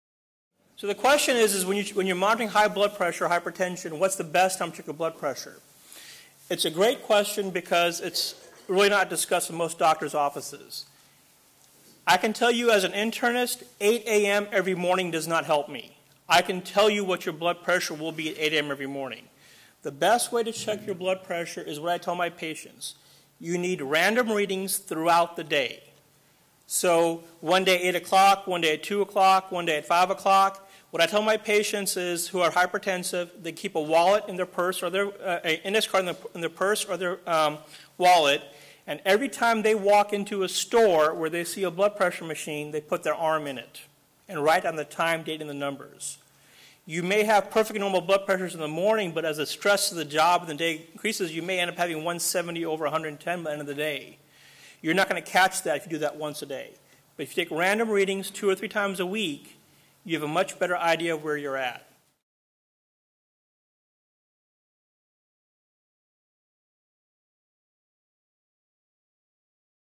When To Check Your Blood Pressure?
What is the best time to check your blood pressure to get the most accurate picture of your health. Checking the same time every day is not always best. Find out why.